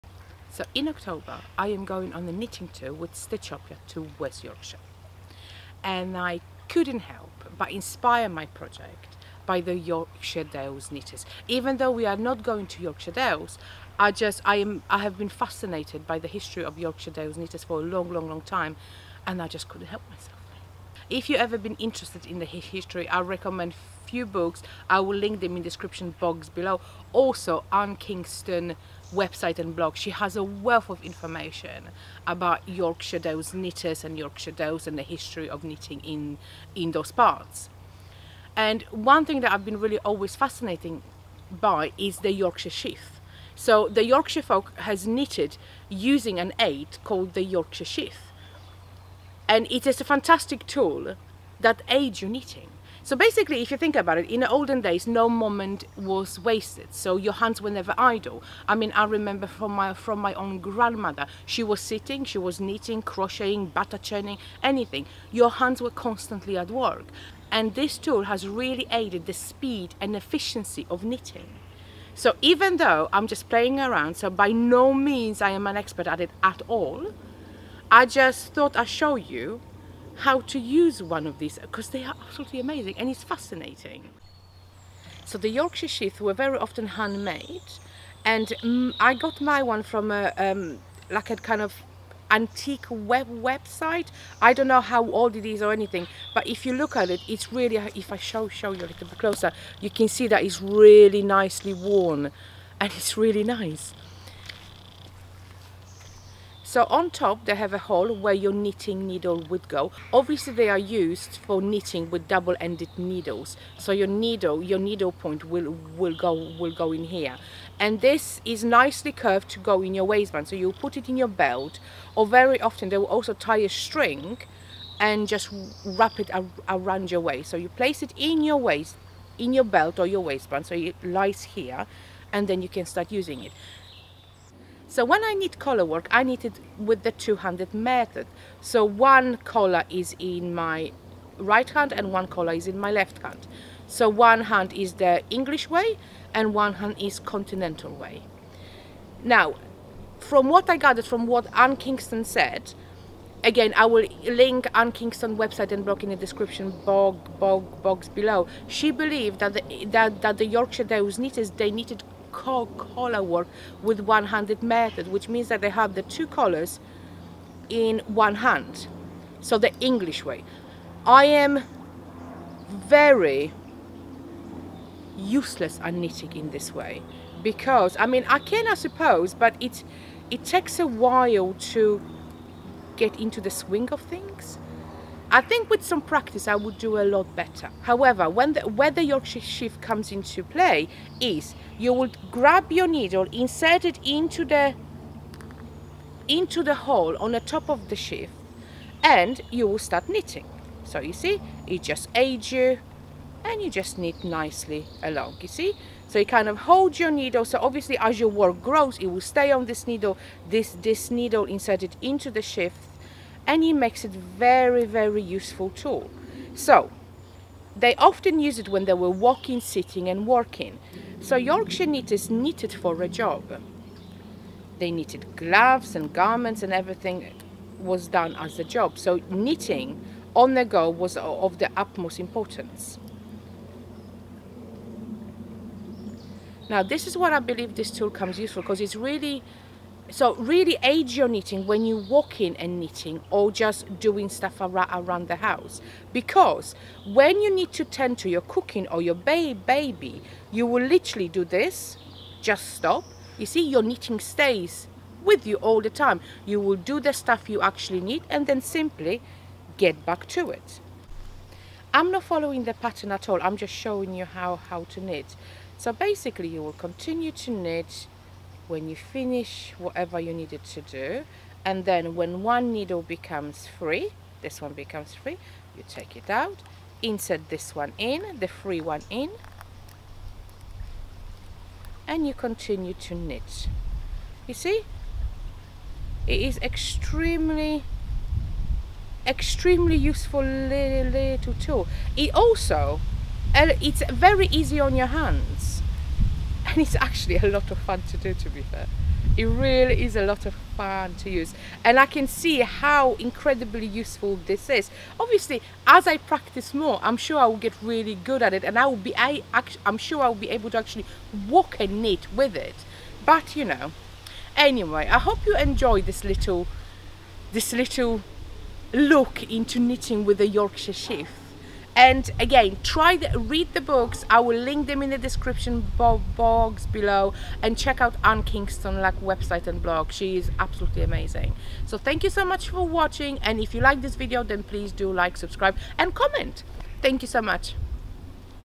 Knitting with Yorkshire Sheath
This is just a small video where I have a go at knitting using the Yorkshire Sheath.
If you are interested in the history of Yorkshire Dales knitters, check out this book and blog:

Anna Kingstone is amazing and has a wealth of information about this subject. Check out her YouTube channel and blog: